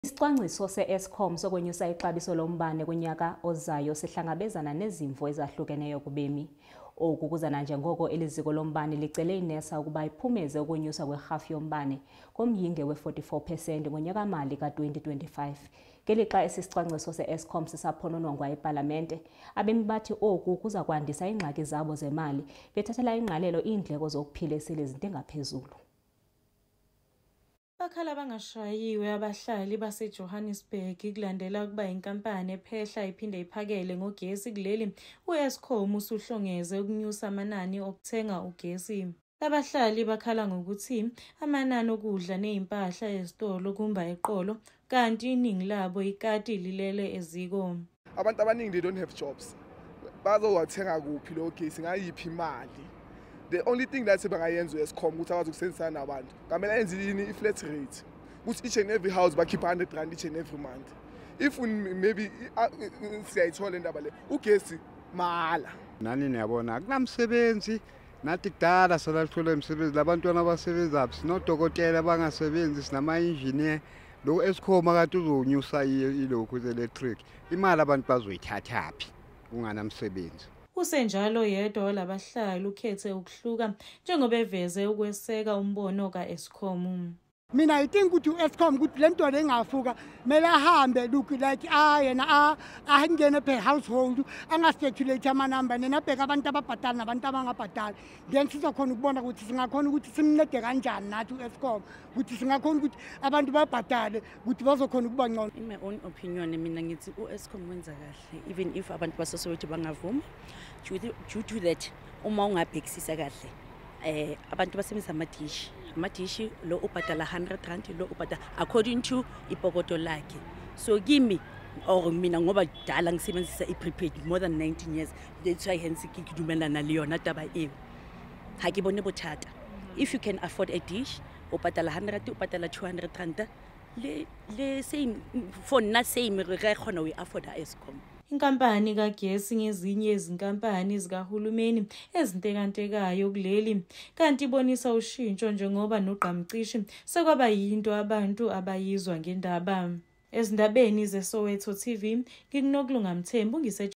SOWETO TV NEWS | l-ESKOM UFUMANA IZIGXEKO NGENJONGO YAYO YOKUNYUSA IXABISO LOMBANE KUNYAKA OZAYO
Isicwangciso seEskom sokunyusa ixabiso lombane nge-44% kunyaka ozayo sihlangabezana nezimvo ezahlukeneyo kubemi.